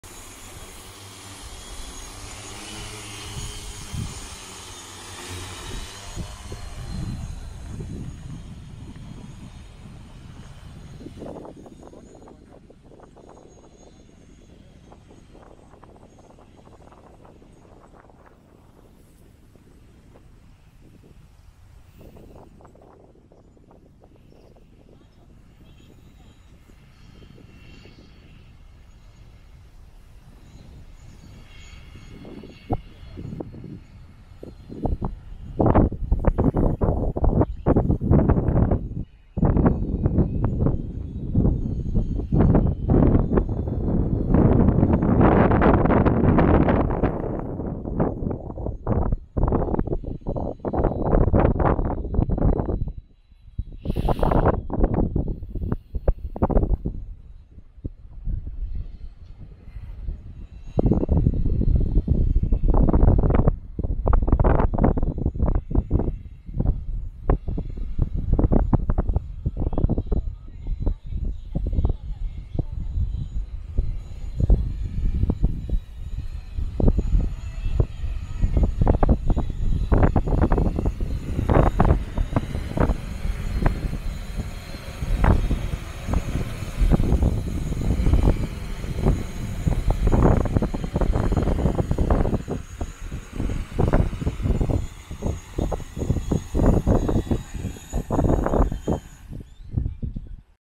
農薬散布ドローン練習風景
農薬散布ドローン５リットル機、納品済みのお客様宅での散布練習風景。